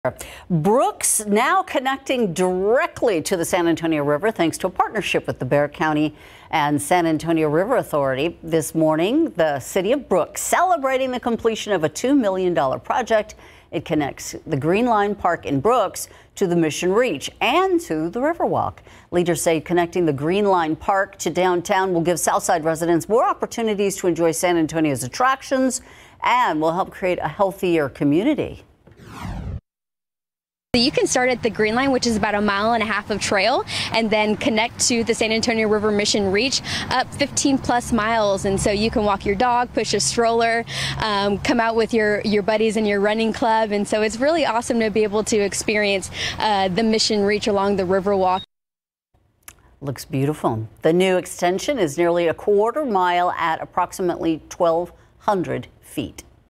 Brooks completes project connecting Greenline Park to downtown SA
Officials on Wednesday celebrated the completion of a $2 million project that connects the Greenline Park at Brooks to the Mission Reach and the River Walk.